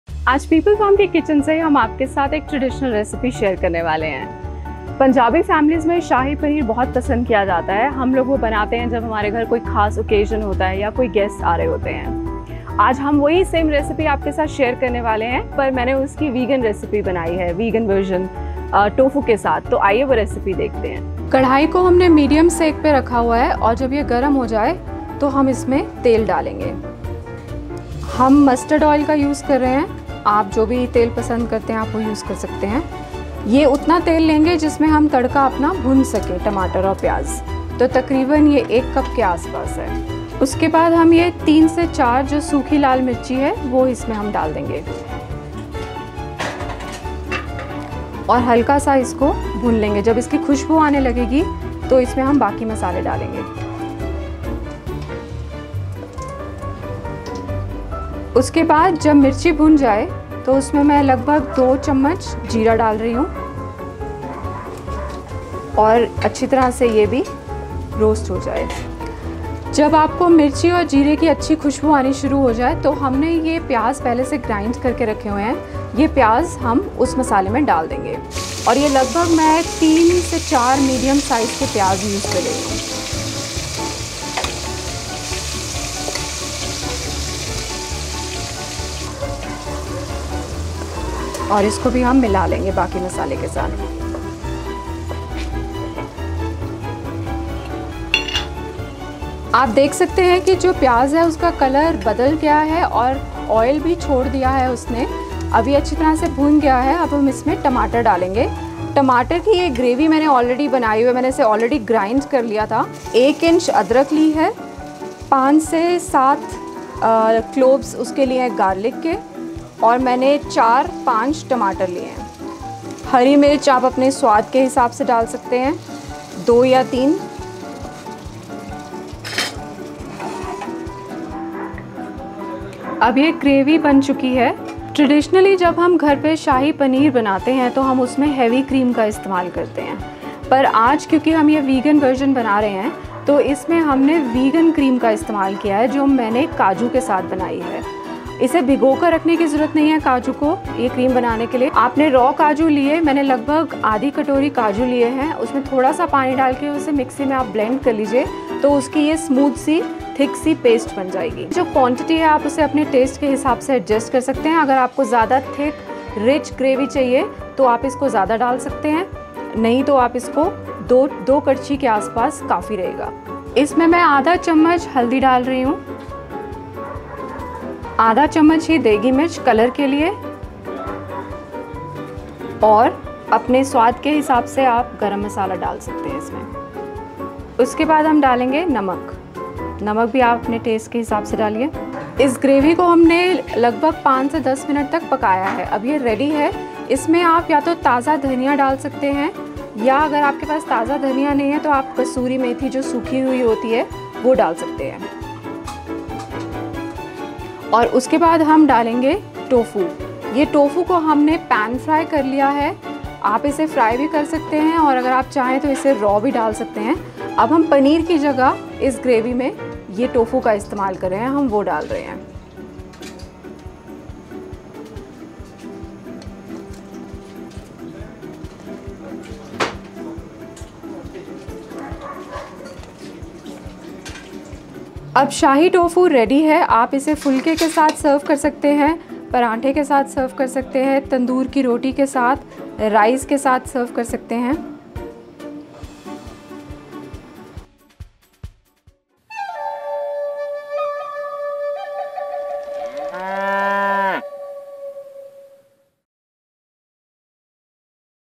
How to make Shahi Paneer without Paneer | Tofu paneer recipe | Tofu sabji
Shahi Paneer is a popular Indian dish among Punjabi families. 'Shahi' means Royal in English and this dish lives up to its name. It's a dish every Punjabi enjoys eating and is prepared on every festival, birthday party, wedding and it is one of the top three ordered dishes at family restaurants.

For those who can't consume dairy or are trying to move away from dairy for health reasons,we have made this recipe of Shahi Tofu which does not use any dairy.